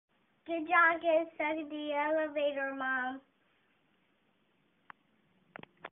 Even 4 year olds are snarky
This is what my hubby sent me when I texted him I was stuck in an elevator at work